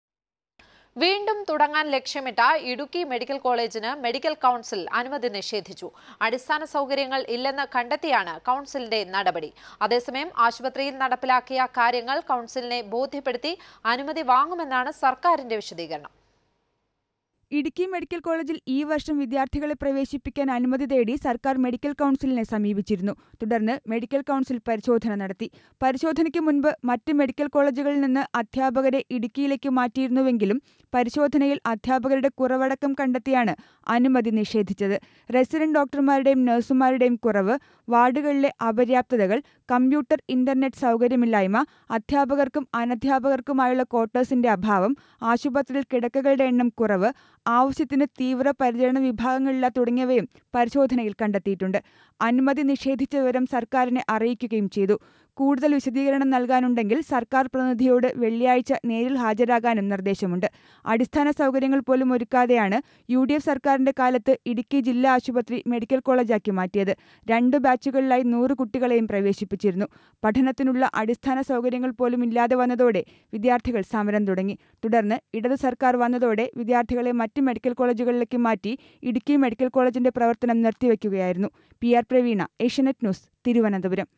MCI denies permission to start Idukki Medical College
The Medical Council of India (MCI) denies permission to start Idukki Medical Colleges